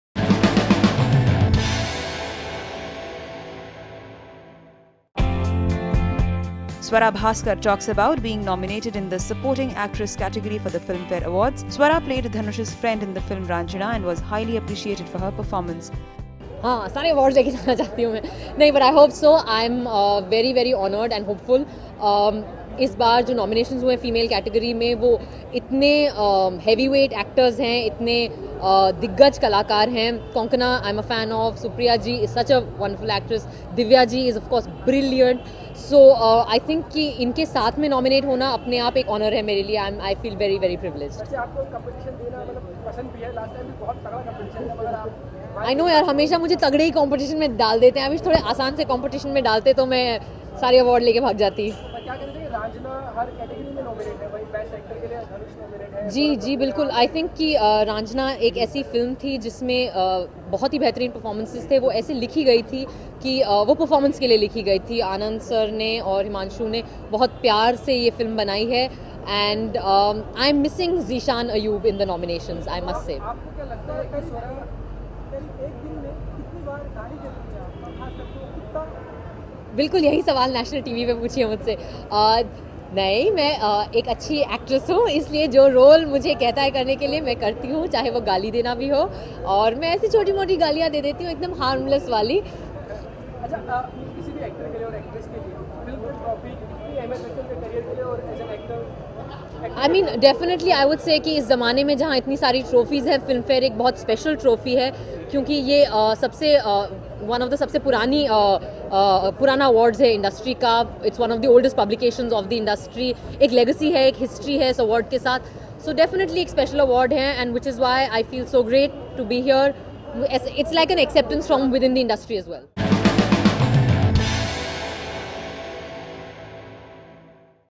Swara Bhaskar nominated for Filmfare award
Swara Bhaskar talks about being nominated in the supporting actress category for the Filmfare awards. Swara played Dhanush's friend in the film "Raanjhanaa" and was highly appreciated for her performance.